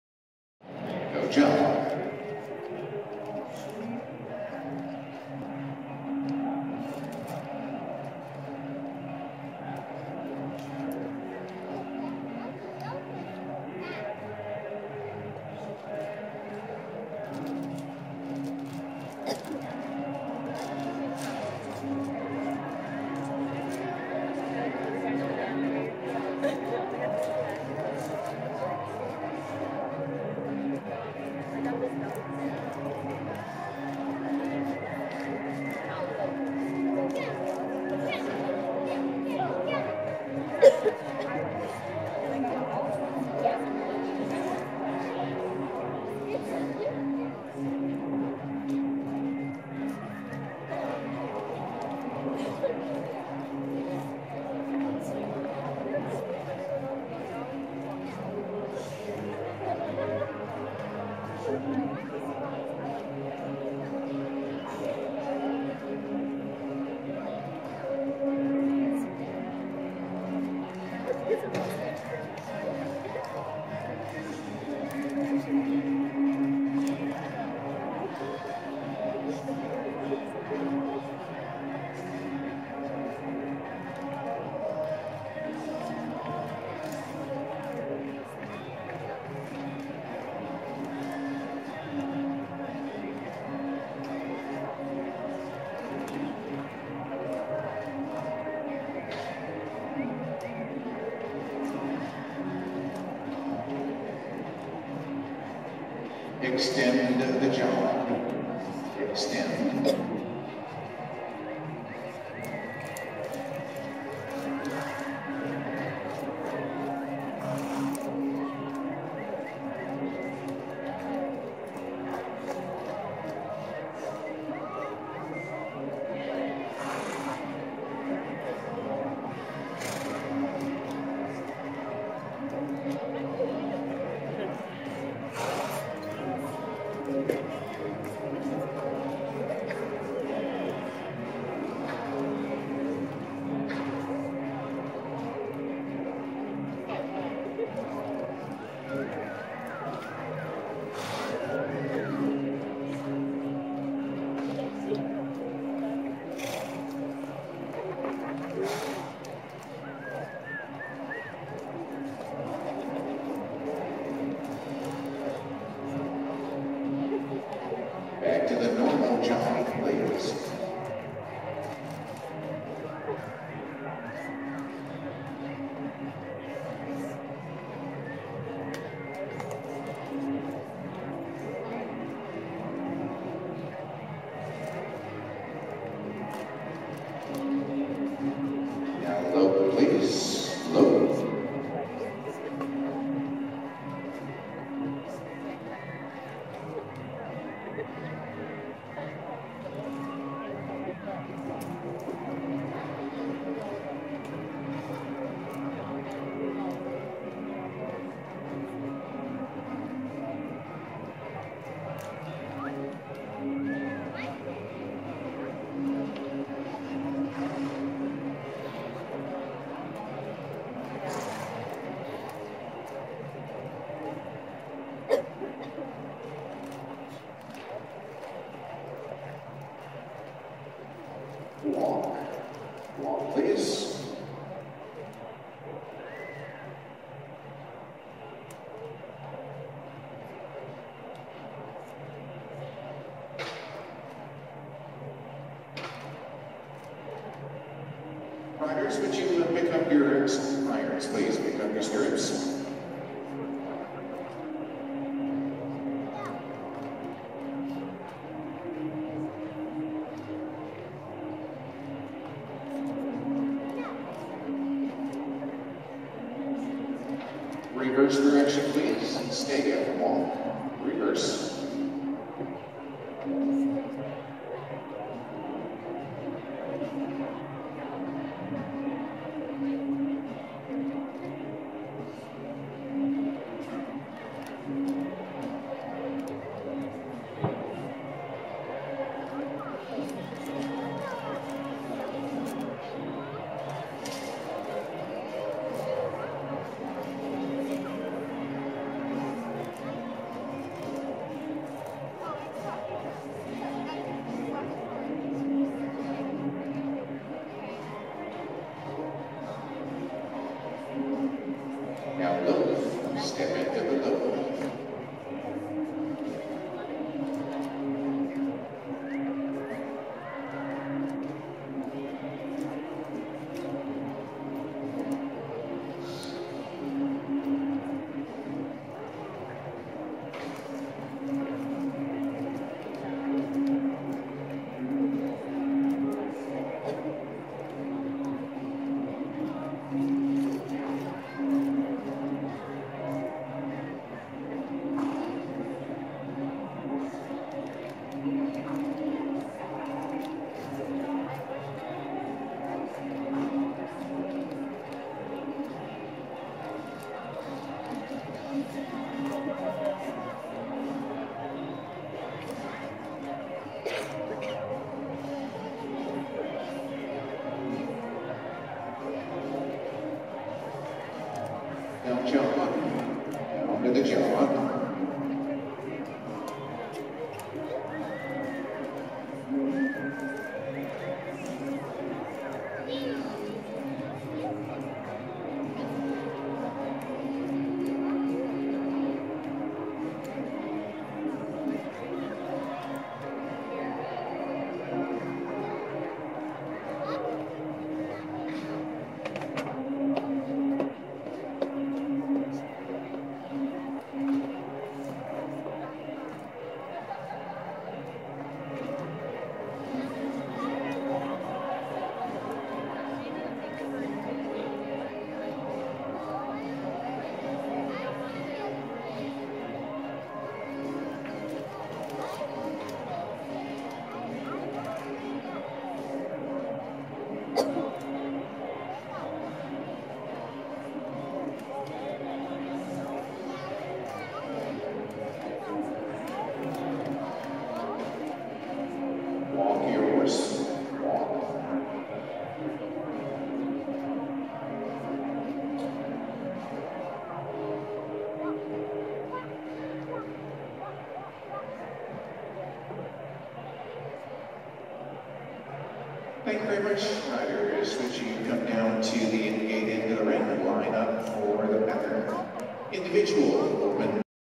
Conner Smith - Individual Open Western Horsemanship - 2018 IHSA Nationals
Conner Smith, a Senior at St. Andrews University in Laurinburg, NC competes in the Individual Open Western Horsemanship class at the 2018 IHSA Nationals in Harrisburg, PA.